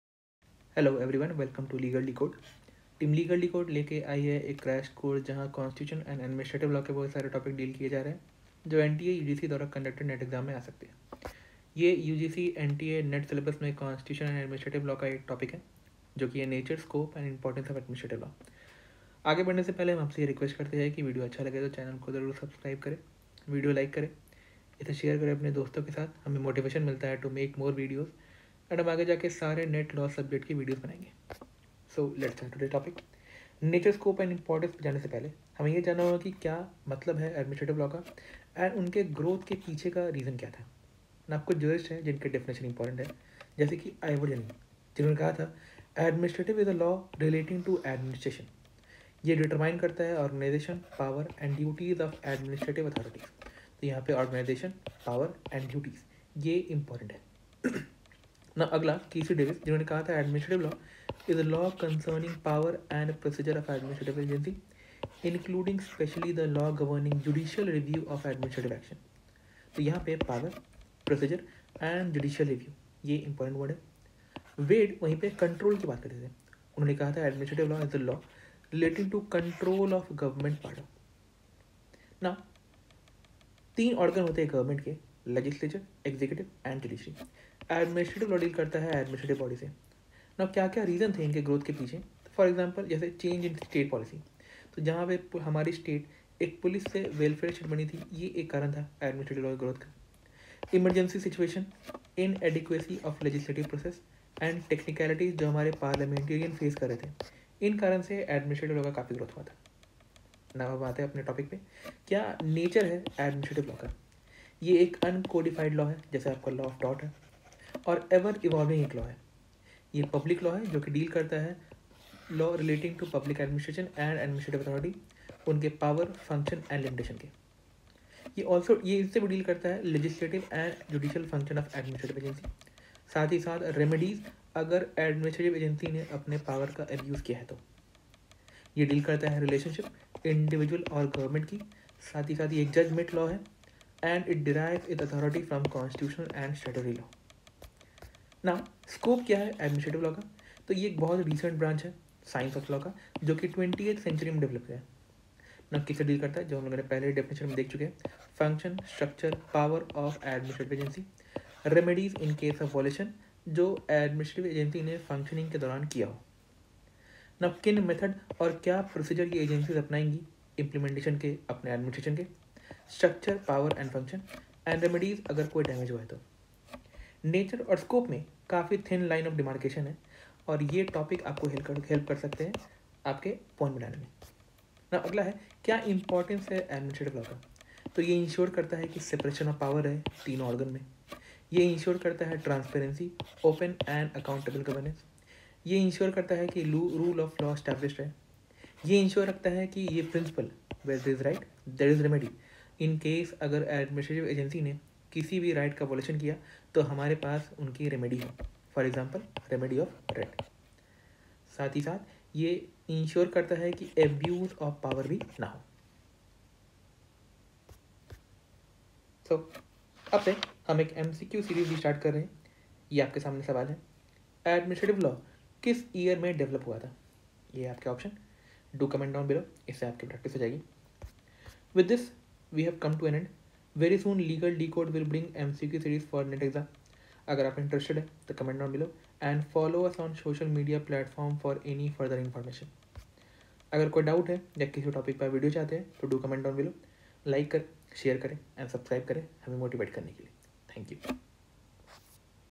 NATURE, SCOPE AND IMPORTANCE OF ADMINISTRATIVE LAW | Constitution of India | CASE LAW | UGC NET
Hello everyone, welcome to LEGAL DECODE.

NTA UGC NET is approaching and Constitution of India contains maximum marks in paper 2 (LAW).
We are presenting Crash Course on Constitution of India where we will be learning important topics that is most important for your exam.
In this video we will be learning NATURE, SCOPE AND IMPORTANCE OF ADMINISTRATIVE LAW and important CASE LAWS.

If you liked the video, please press the LIKE button, SHARE and COMMENT down bellow.